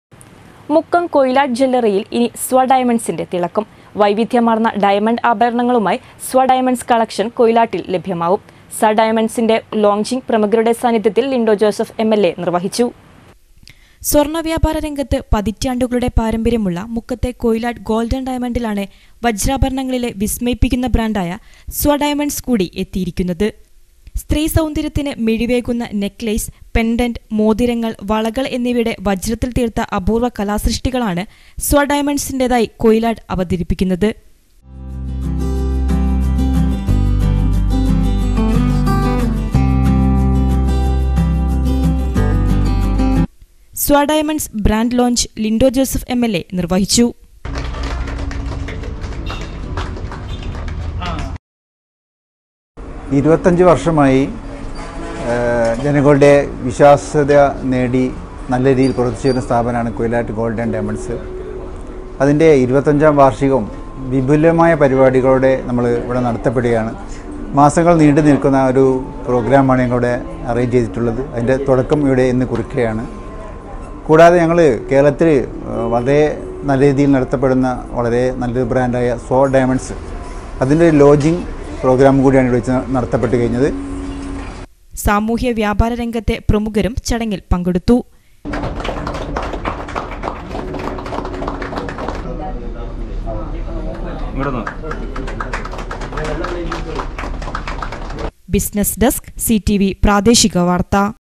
മുക്കം കൊയിലാട്ട് ജ്വല്ലറിയിൽ ഇനി സ്വാ ഡയമണ്ട്സിന്റെ തിളക്കം. വൈവിധ്യമാർന്ന ഡയമണ്ട് ആഭരണങ്ങളുമായി
മുക്കം കൊയിലാട്ട് ജ്വല്ലറിയിൽ ഇനി സ്വാ ഡയമണ്ട്സിന്റെ തിളക്കം. വൈവിധ്യമാർന്ന ഡയമണ്ട് ആഭരണങ്ങളുമായി 'സ്വാ'
ഡയമണ്ട്സ് കളക്ഷൻ കൊയിലാട്ടിൽ ലഭ്യമാവും.
'സ്വാ'  ഡയമണ്ട്സിന്റെ ലോഞ്ചിംഗ് പ്രമുഖരുടെ സാന്നിധ്യത്തിൽ ലിന്റോ ജോസഫ് എംഎൽഎ നിർവഹിച്ചു.